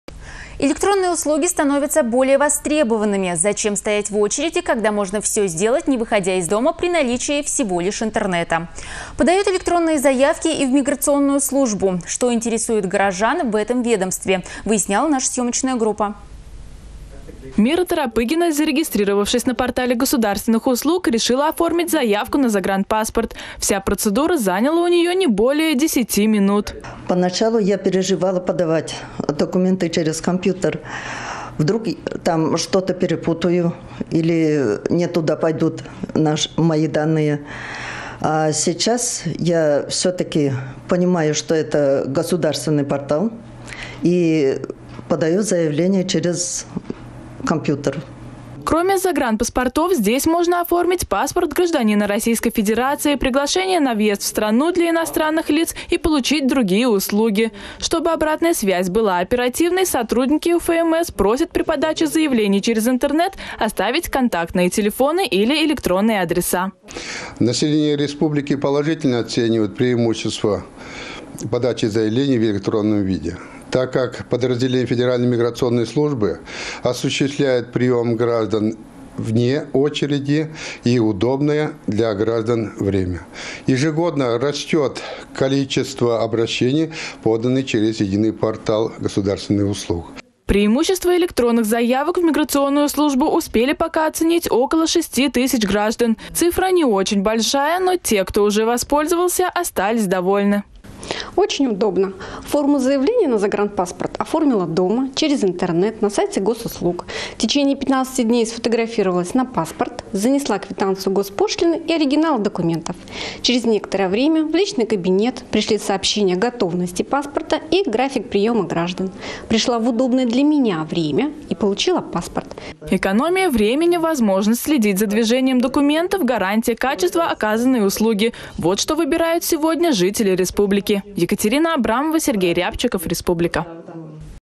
Электронные услуги становятся все более востребованными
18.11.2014
Зачем стоять в очереди, когда можно все сделать не выходя из дома, при наличии всего лишь сети Интернет. Подают электронные заявки и в миграционную службу республики.